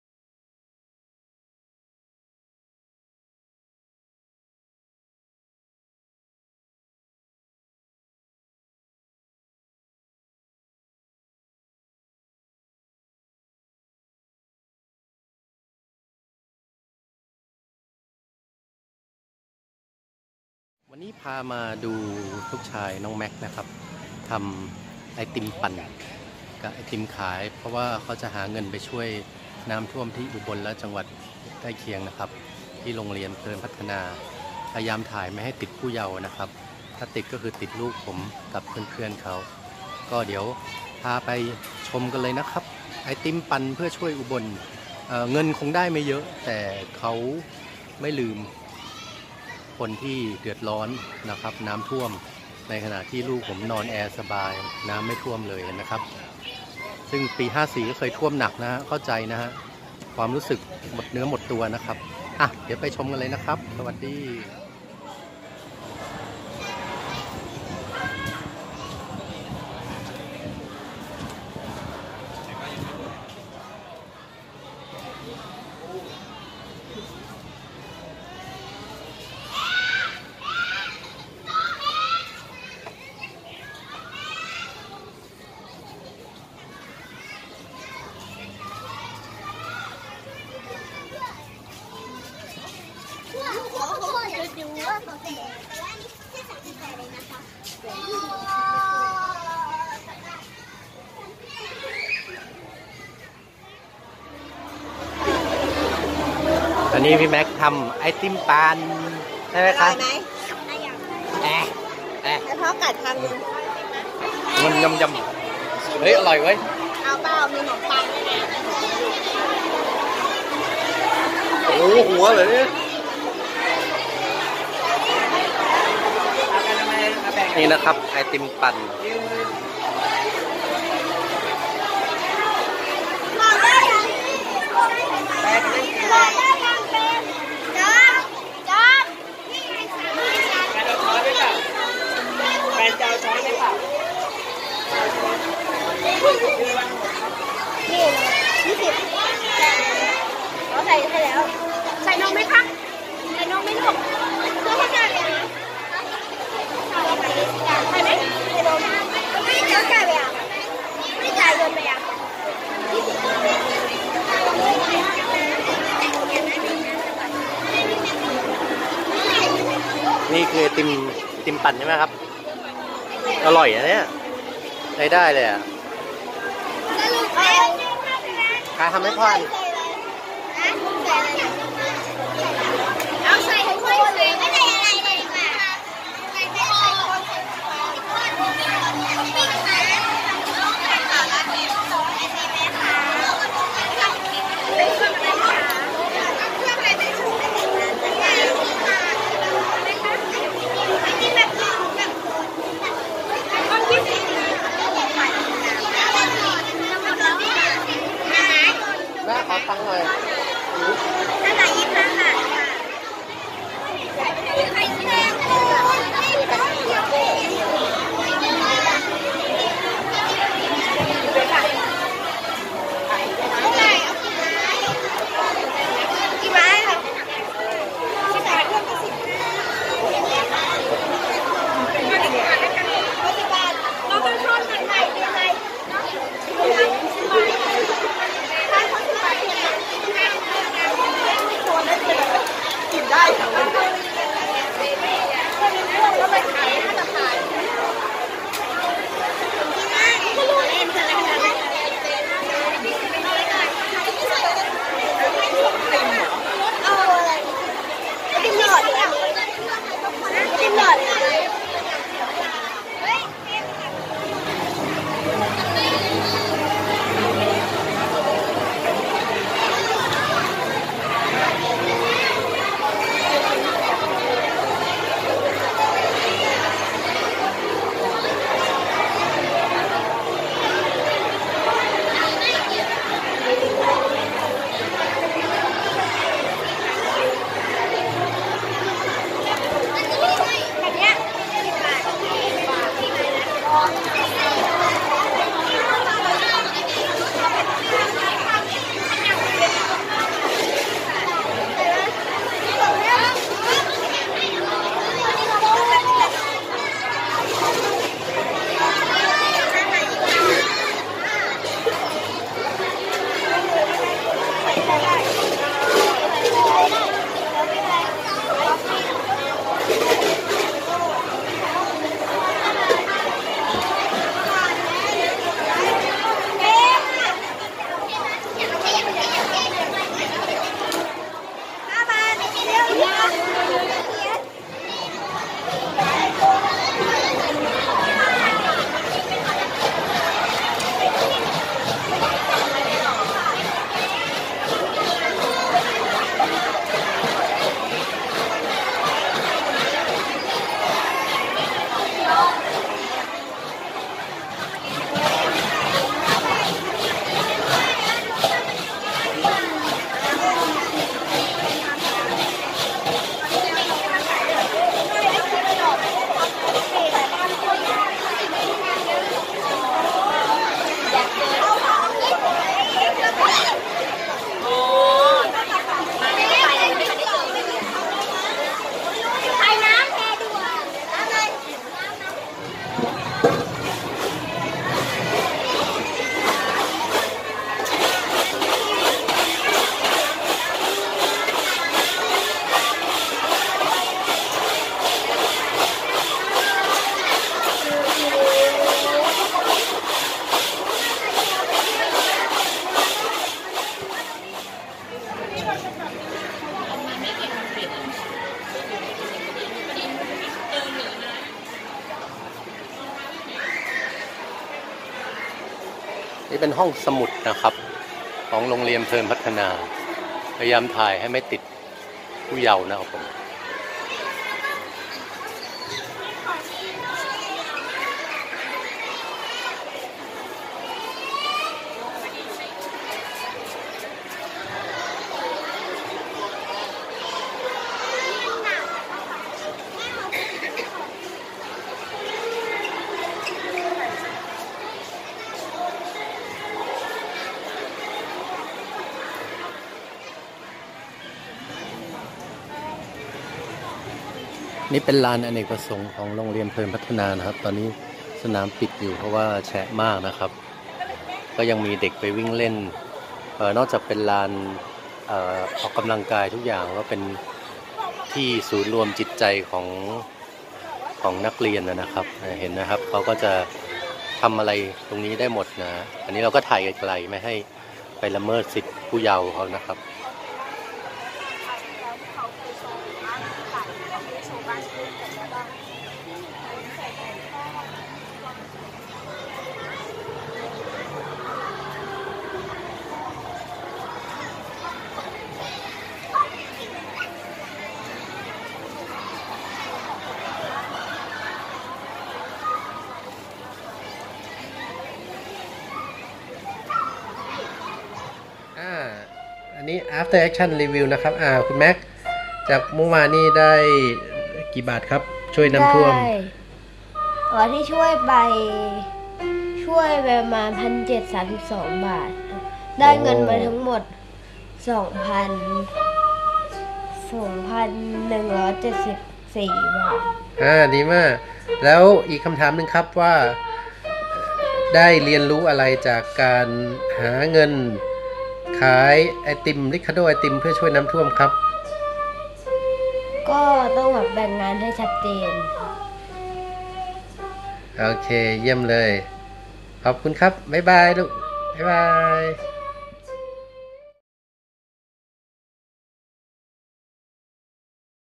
Max แทน เปรม
นักเรียนโรงเรียนเพลินพัฒนา ช่วยกันหาเงิน
เพื่อให้เห็นถึงความยากลำบากของคนไทยที่ตกทุกข์ครับ
Max แทน เปรม หารายได้ได้ 1,732.00บาท
แม้จะไม่ใช่เงินจำนวนมากมายแต่สิ่งที่ได้คือความภูมิใจของ นักเรียน
และความเห็นใจผู้อื่น คนไทยด้วยกันไม่แบ่งสีแบ่งฝ่าย
คลิปนี้ทำขึ้นมาเพื่อแบ่งปัน ไม่มีเหตุผลทางพาณิชย์แต่อย่างใด